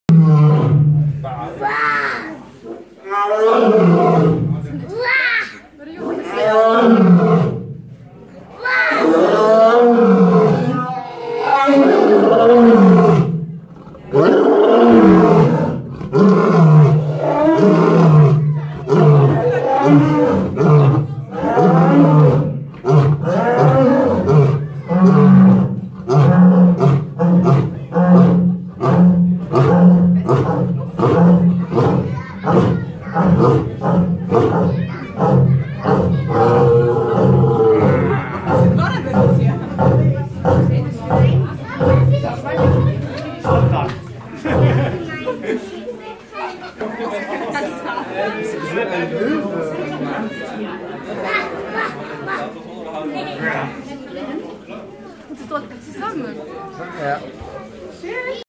Lion Roaring - Barbary lion
The Barbary lion is considered a population of the African lion subspecies Panthera leo leo. This population is regionally extinct in North Africa. It inhabited the Atlas Mountains and was therefore also known as the Atlas lion.